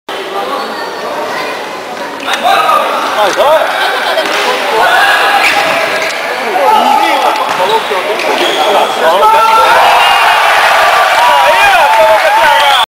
GOLAÇO DO PROF MARCELO HENRIQUE
Gol feito pelo professor Marcelo Henrique num amistoso na FACEX. O goleiro do time adversário saiu jogando, perdeu a bola, que caiu no pé direito do professor, e com o esquerdo encobriu o goleiro do meio da quadra. Detalhe: a bola passou entre o travessão e outro defensor (sorte e talento.rsrsrsr)